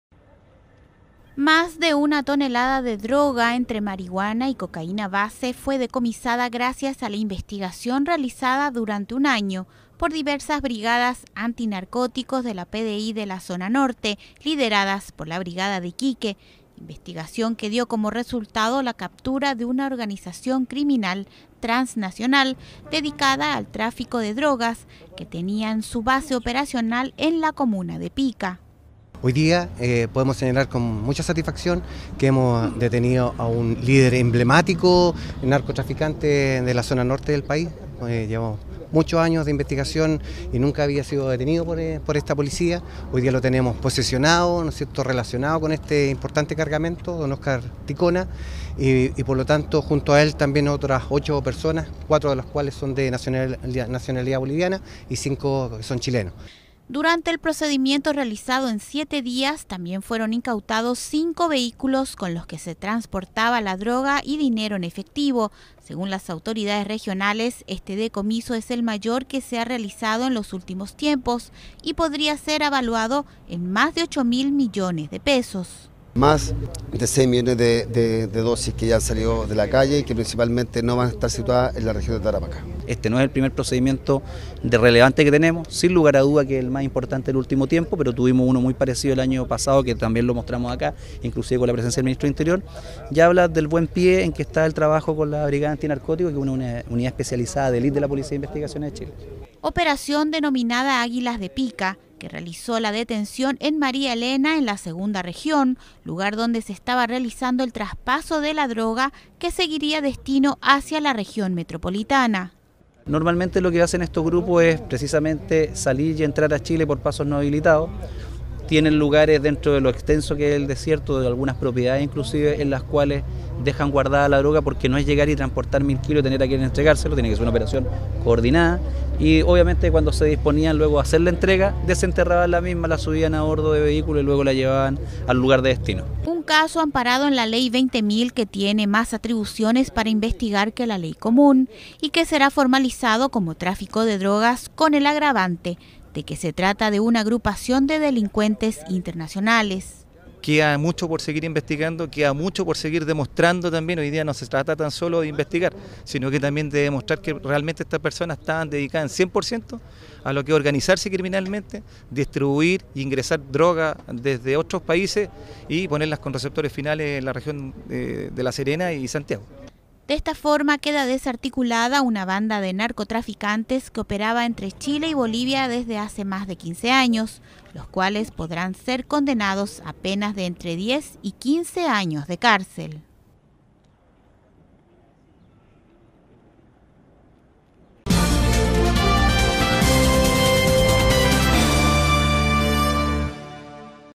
DECOMISAN MÁS DE UNA TONELADA DE DROGA EN TARAPACÁ - Lengua de Señas
(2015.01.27) Más de una tonelada de droga entre marihuana y cocaína base, fue decomisada gracias a la investigación realizada durante un año por diversas Brigadas Anti Narcóticos de la Zona Norte, lideradas por la Brigada de Iquique.